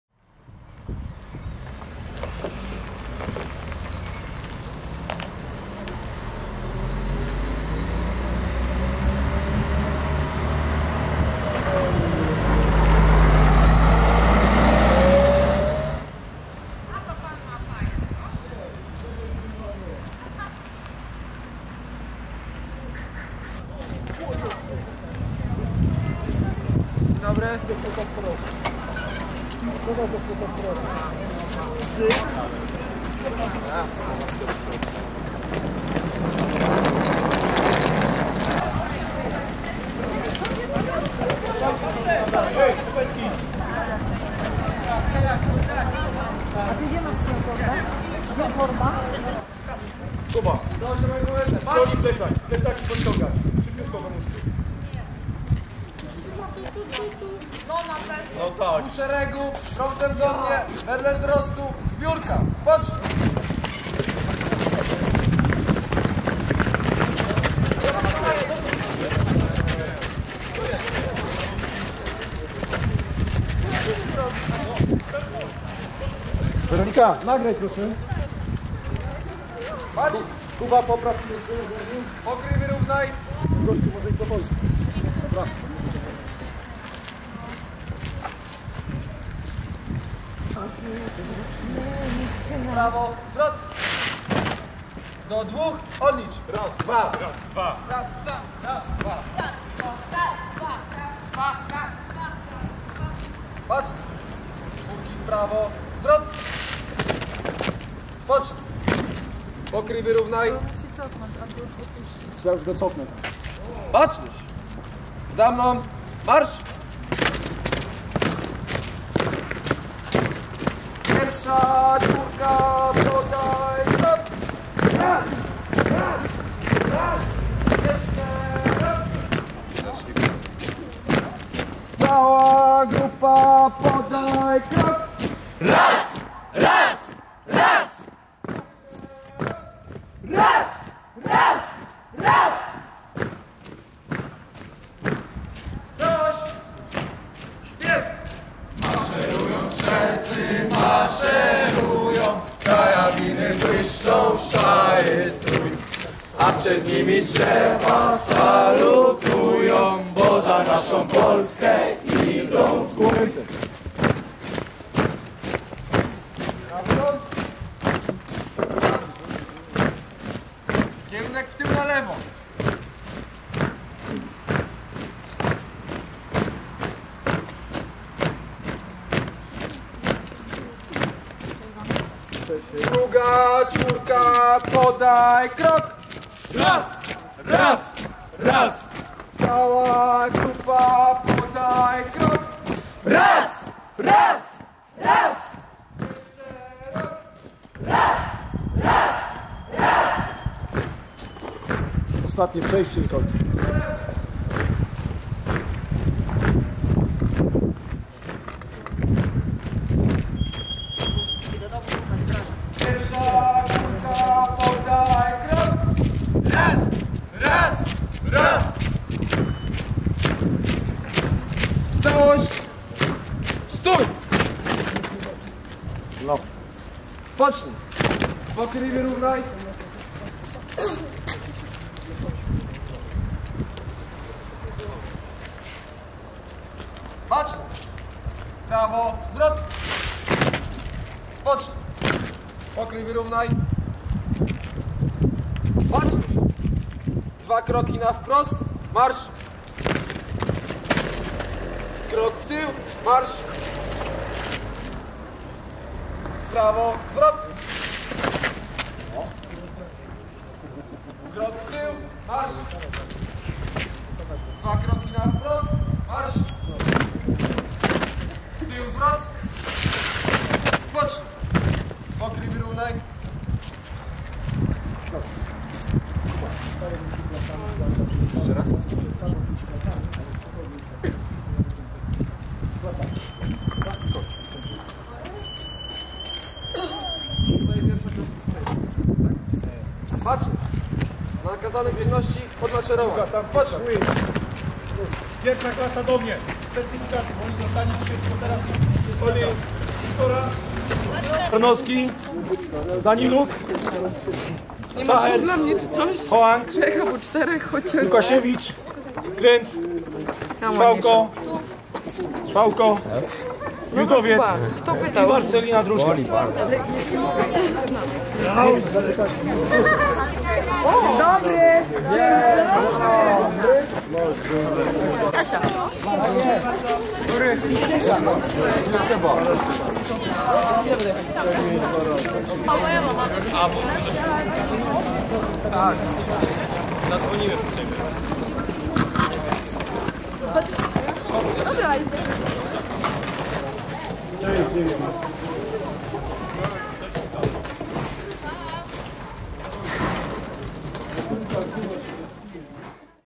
ZAKOŃCZENIE OBOZU ZIMOWEGO KLAS MUNDUROWYCH
Dnia 18 lutego powróciła pierwsza grupa uczniów z  zimowego obozu  szkoleniowego klas pierwszych i drugich mundurowych (wojskowych, strażackich, policyjnych) w Ośrodku Szkoleniowo Sportowym LZS „JAGNA” w Nowej Bystrzycy.